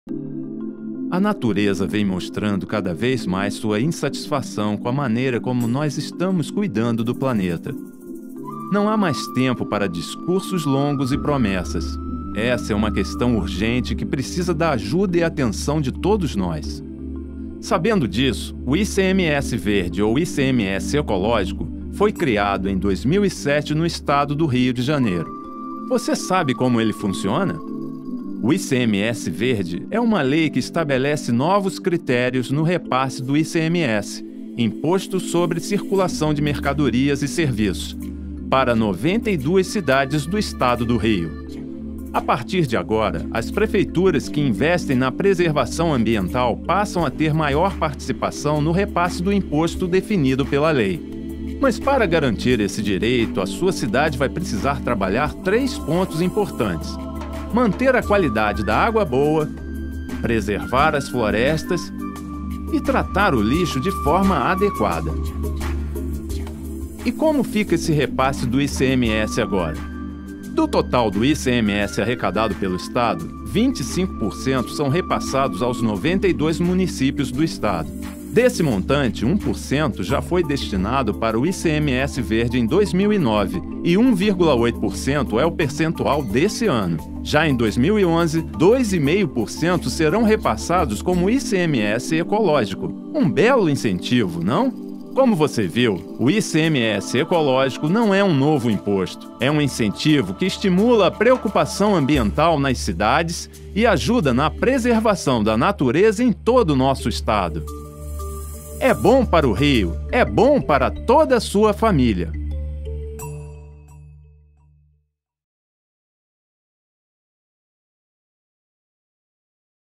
Sérgio Cabral - Conheça o ICMS Verde
O ICMS Verde não é um novo imposto, mas sim uma maneira nova de dividir a arrecadação para beneficiar cidades que tenham projetos de proteção ambiental.

Assista ao vídeo e saiba um pouco mais sobre o que é o ICMS Verde, criado no governo Sérgio Cabral, e como ele impacta a sua vida e de sua cidade.